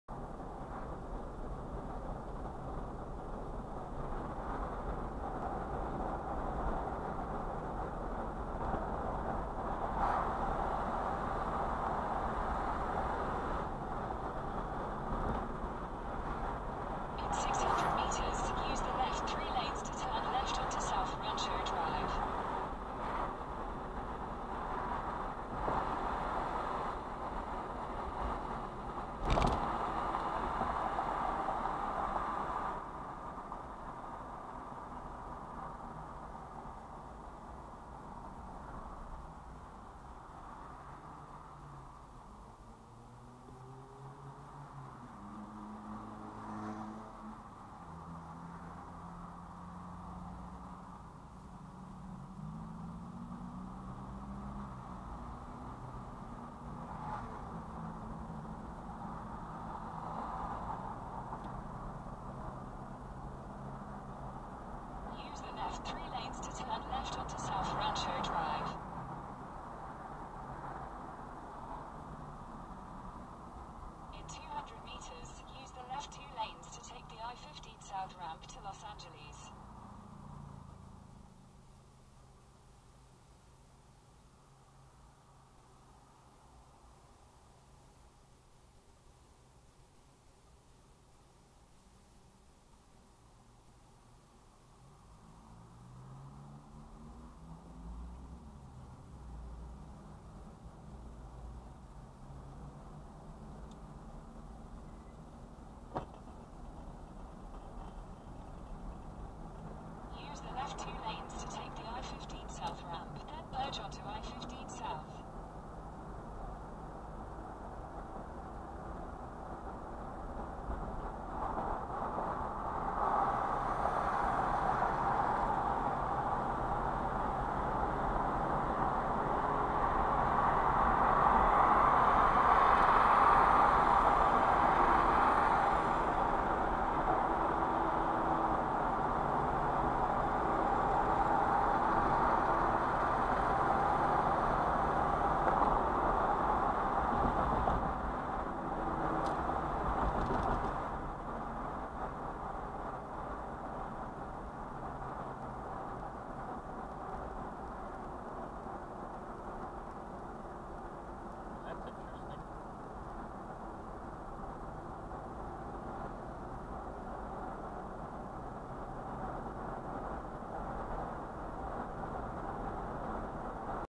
[2019-05-02] [Novel Gantry Frames] [Video alongside synchronised phone screen recording]
Possibly the first frames of the lighted gantry at United States southbound interstate route 15, before exit 39 (Nevada, Las Vegas, Spring Mountain Road)